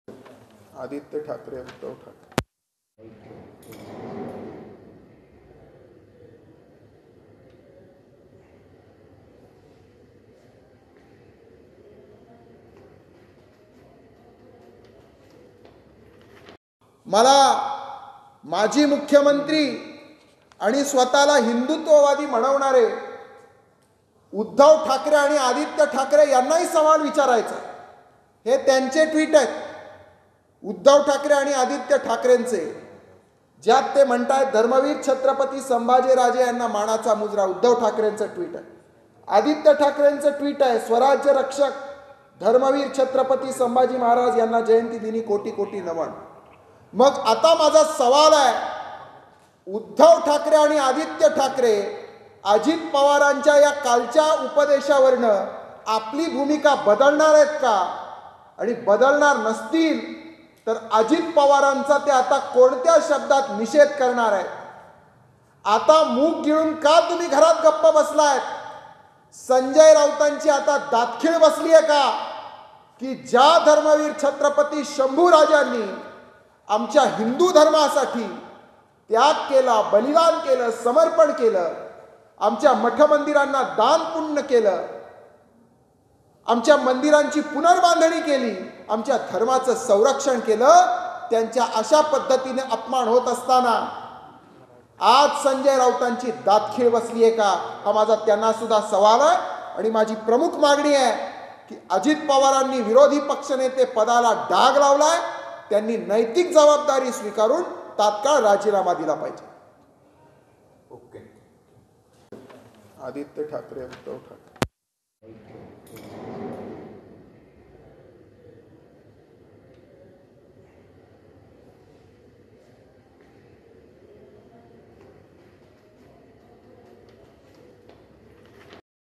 NASHIK | अजित पवार यांनी विरोधी पक्ष नेते पदाला डाग लावला आहे - तुषार भोसले
-अजित पवार म्हणतात की, 'छ्त्रपती संभाजीराजे हे धर्मवीर नव्हते' -विरोधी पक्षनेते अजित पवार यांनी जनतेच्या भावना दुखावण्याचा प्रयत्न केला -त्यांनी तात्काळ विरोधी पक्षनेते पदाचा राजीनामा द्यावा -त्यांनी छ्त्रपती संभाजी राजे यांचा अपमान केल्यामुळे या पदावर राहण्याचा नैतिक अधिकार अजित पवार यांना नाही -असं म्हणण्याचा अधिकार अजित पवार यांना दिला कुणी ? -अजित पवार यांनी तातडीने माफी मागा किंवा राजीनामा द्या -अजित पवार यांचे 14 मे 2019 चे ट्विट दाखवले -अजित पवार 2019 पर्यंत संभाजी महाराज यांना धर्मवीर मानायला तयार होते-मात्र आता 2022 मध्ये कशी त्यांना उपरती आली ? -माजी मुख्यमंत्री उद्धव ठाकरे आणि आदित्य ठाकरे यांनाही प्रश्न विचारायचा आहे..-उद्धव ठाकरे आणि आदित्य ठाकरे आपली भूमिका बदलणार का-की ते अजित पवार यांचा निषेध करणार का -संजय राऊत यांची दातखीळ बसली आहे का? -संभाजी महाराज यांनी धर्मासाठी बलिदान दिले -अजित पवार यांनी विरोधी पक्षनेते पदाला डाग लावला

. BYTE : तुषार भोसले भाजप अध्यात्मिक आघाडी अध्यक्ष